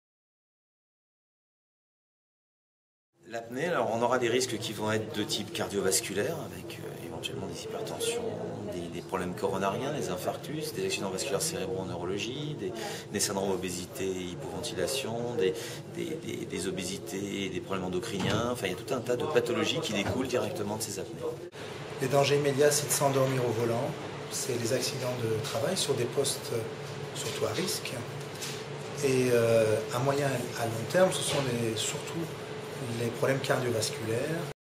Ronflement et Apnée: quels sont les dangers?
Ronflement et Apnée: quels sont les dangers? Une réponse des Dr. Bruno Navailles et Dr. Darius Abedipour, chirurgiens ORL | Somnoforum 2011